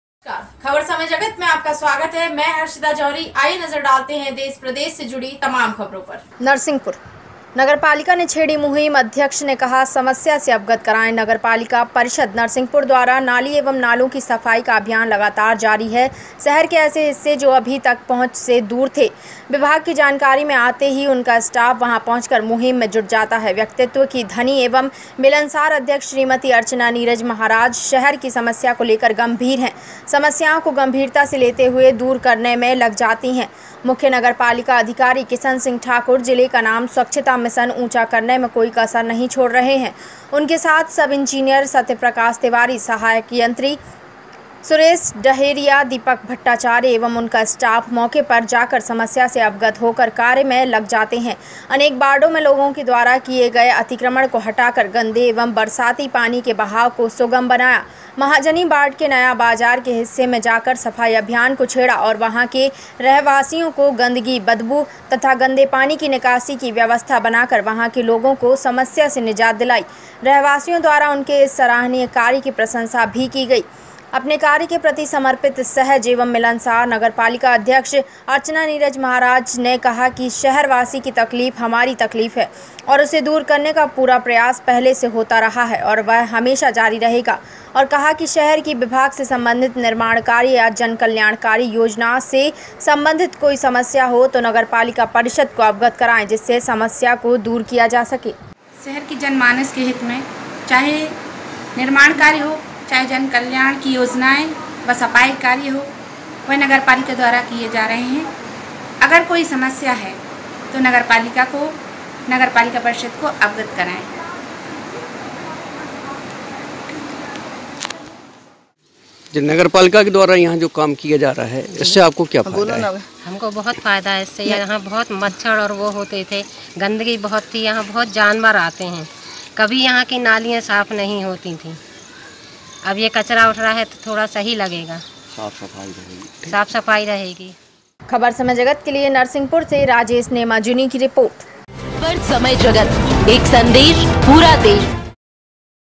नरसिंहपुर - नगर पालिका ने छेड़ी मुहिम अध्यक्ष ने कहा समस्या से अवगत कराएं
नरसिंहपुर समय जगत राजेश  नेमा जूनी